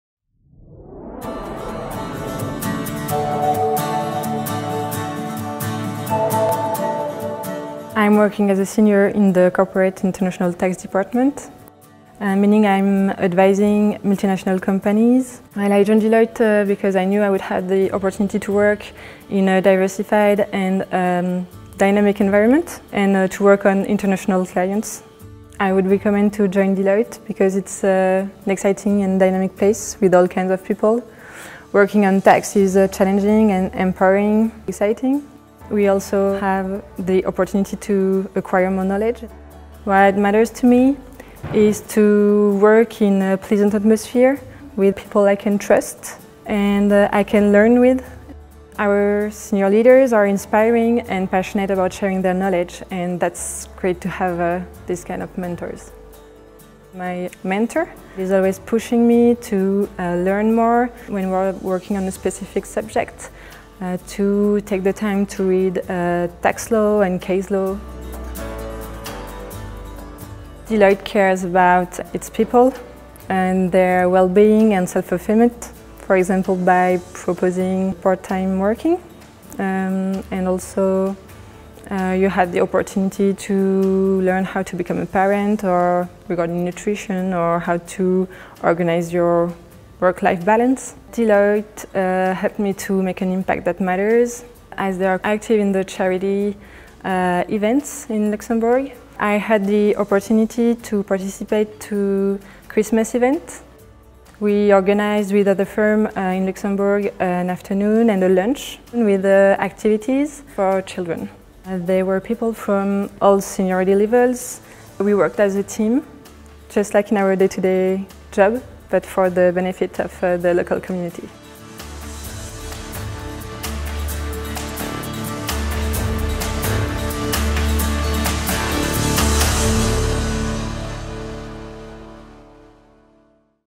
Life at Deloitte. Soulaf Boukbiza's story | About Deloitte
Dynamic, diversified, challenging, empowering, and exciting are some of the words Soulaf uses to describe what it is like to work in Business Tax at Deloitte Luxembourg. What matters to her is well-being, working with people she admires, and senior leaders who are passionate about learning and sharing their knowledge. Soulaf finds that as a Senior at Deloitte, and taking part in charity events in her community, she is fulfilling another passion, making an impact that matters.